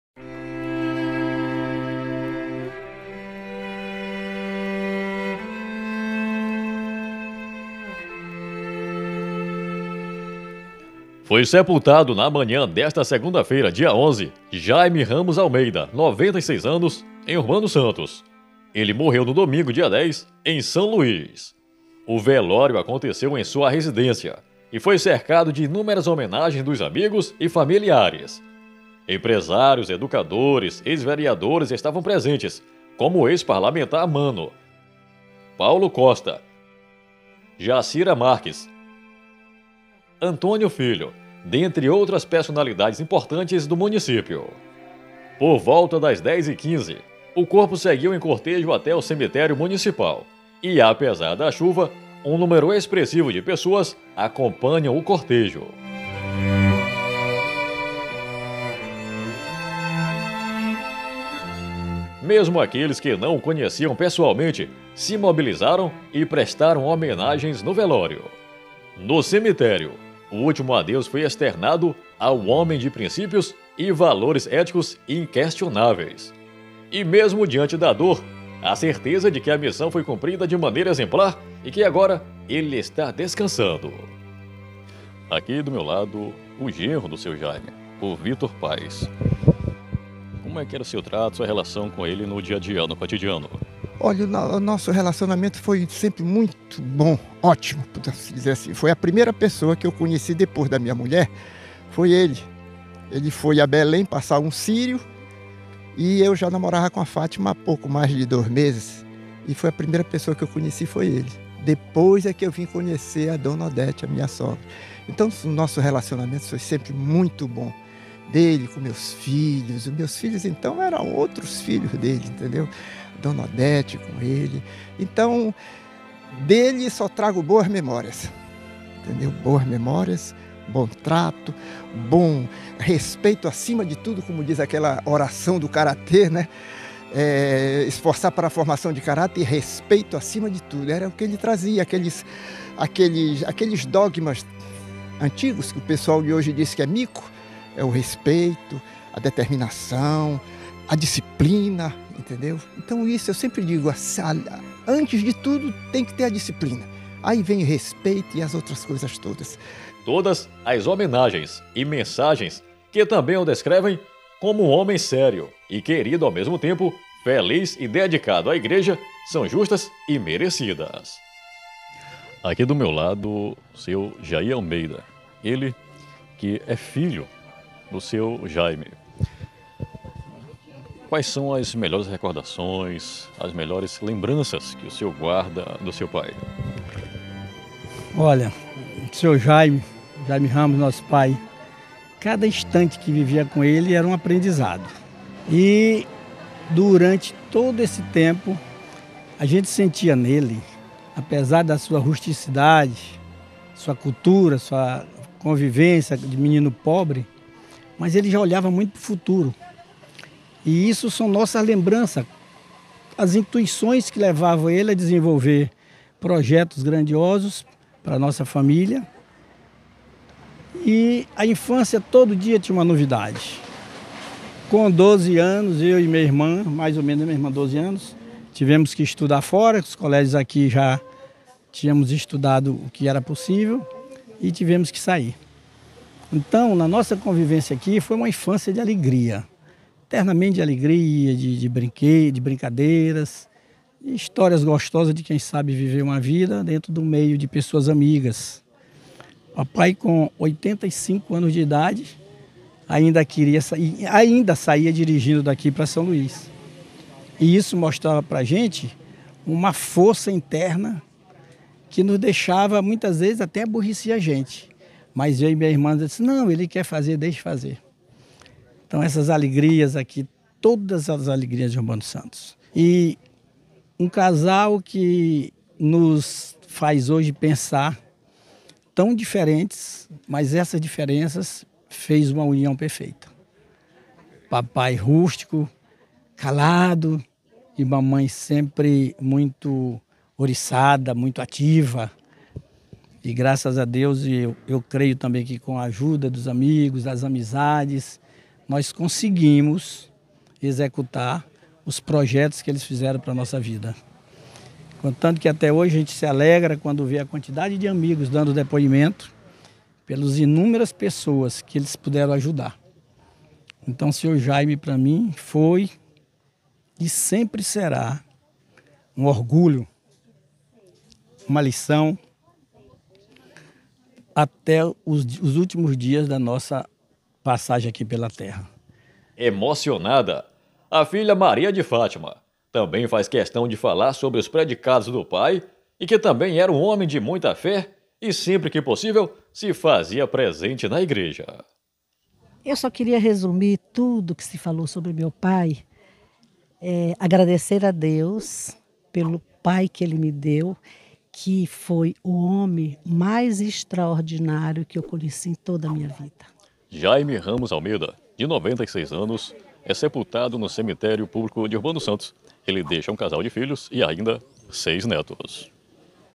Urbano Santos se despede de Jaime Almeida, de 96 anos!
Jaime Almeida, deixa um casal de filhos e 06 netos.